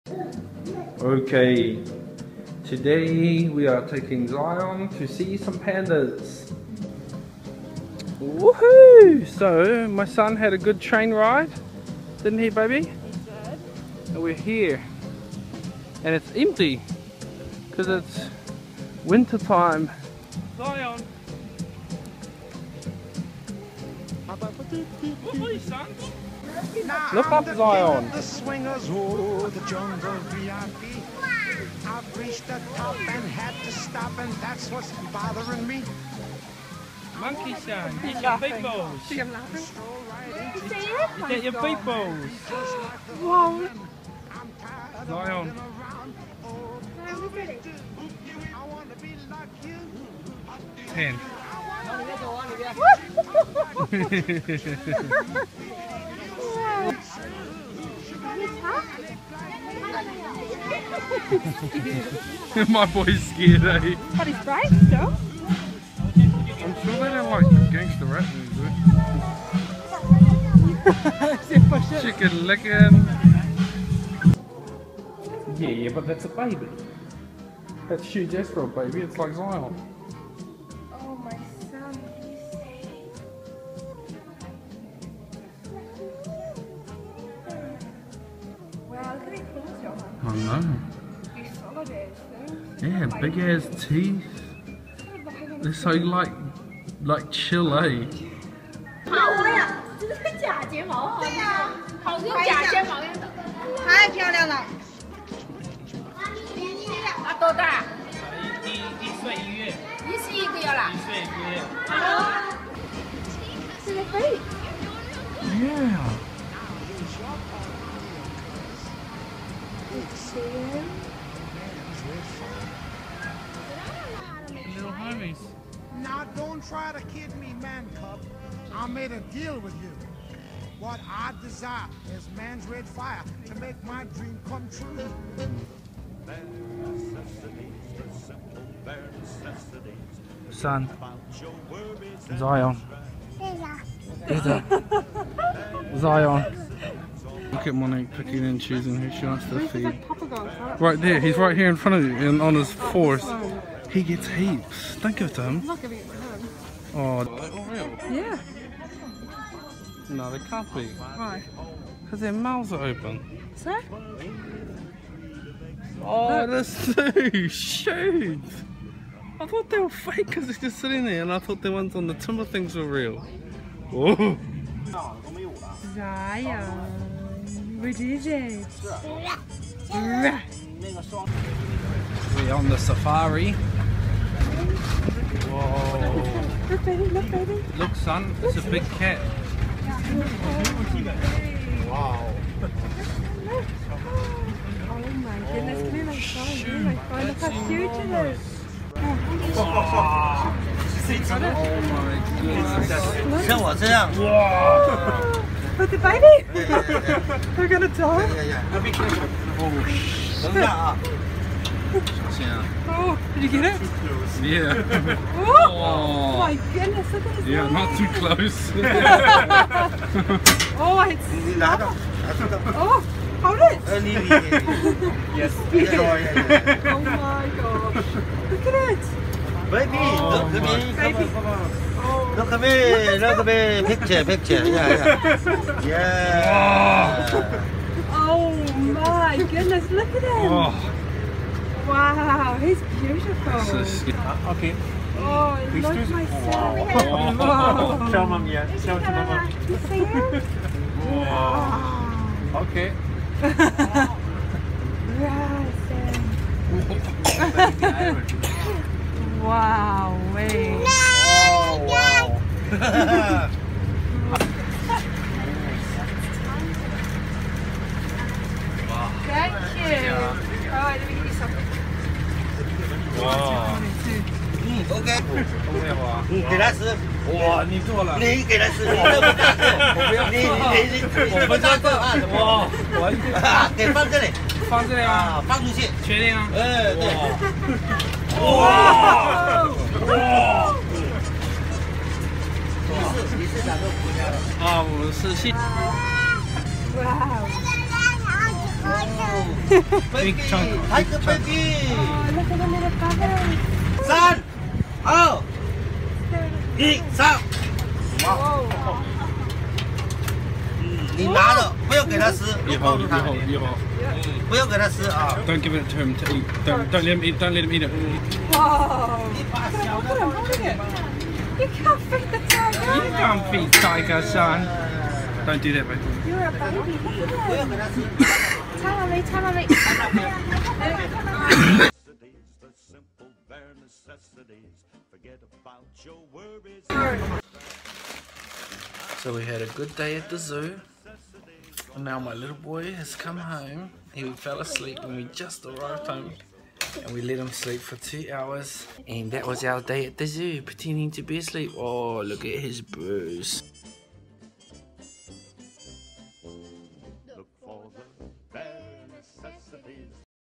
China safari Zoo: Zion trying to feed the cubs.
We had an awesome day at the Shenzhen Safari Park Zoo. We saw a lot of animals but the highlight was definitely seeing and feeding the Tigons. they were truly incredible. The camera doesn't do the size any justice.